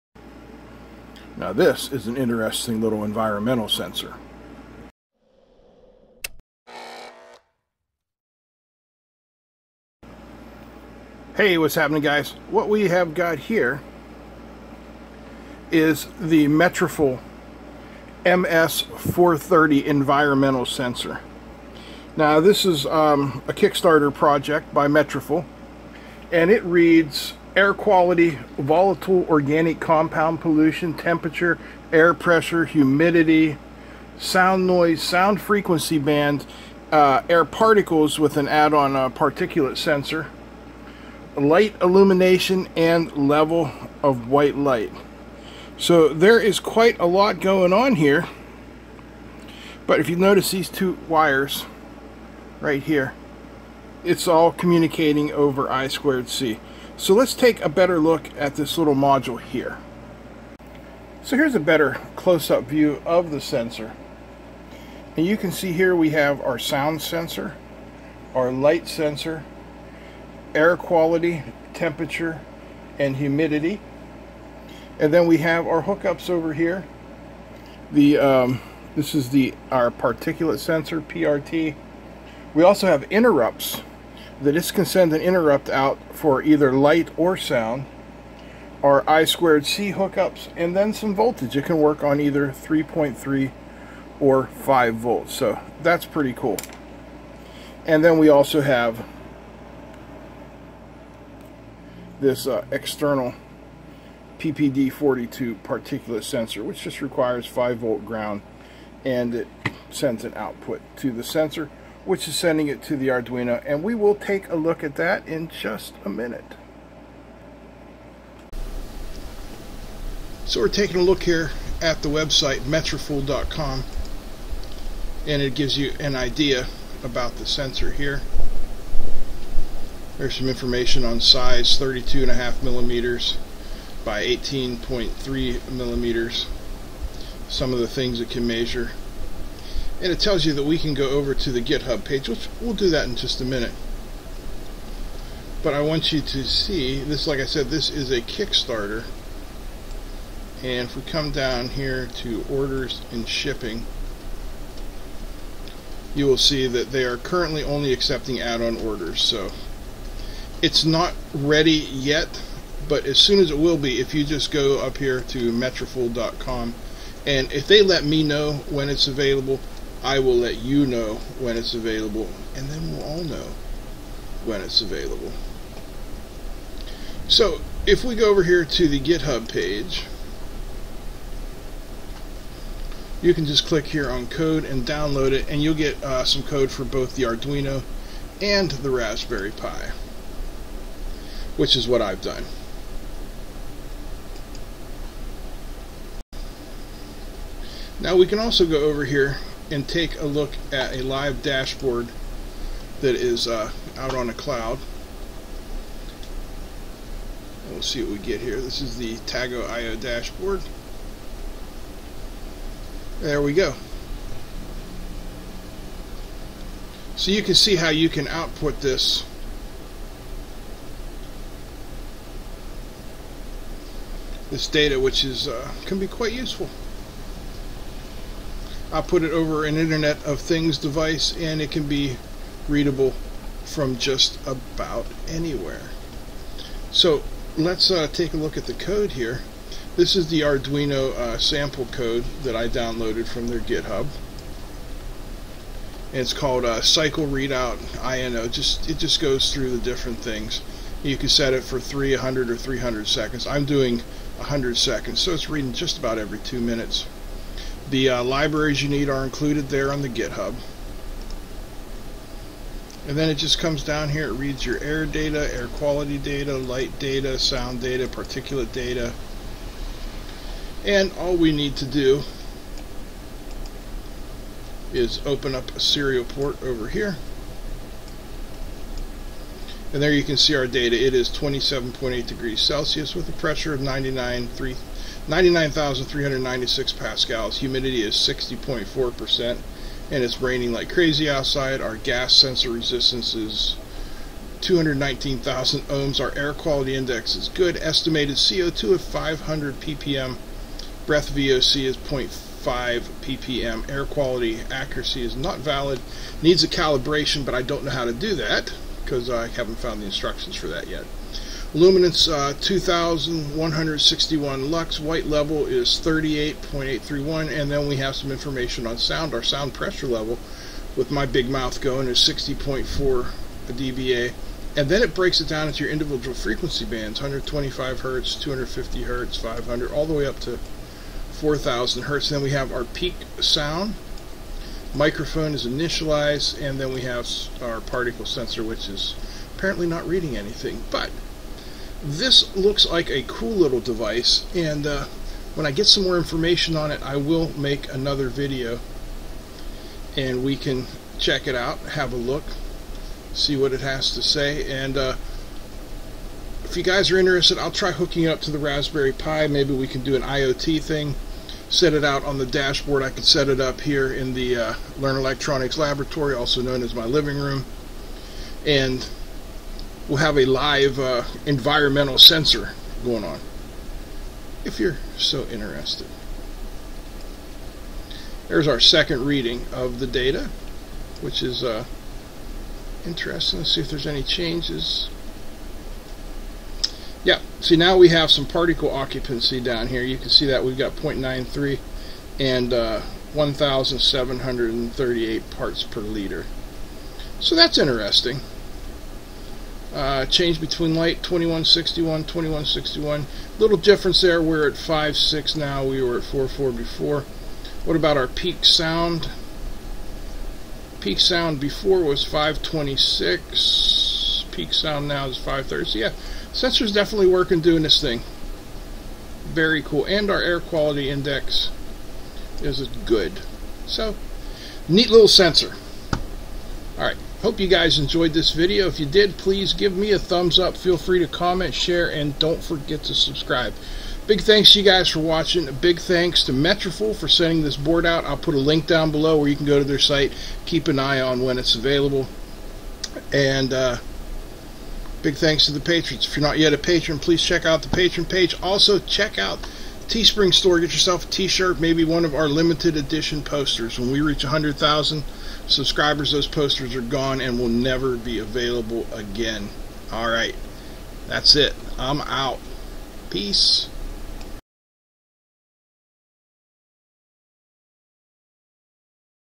Metriful MS430 Environmental Multi Sensor for Arduino and RasPi
CyberCityCircuits - Electronic kits, subscription boxes, tools and components.

In this video we take a first look at the MS430 environmental sensor from Metriful. This is a kickstarter project. This I2C sensor reads sound, light, temperture, pressure, humidity, VOC and particulates. It works with both Arduino and Raspberry Pi.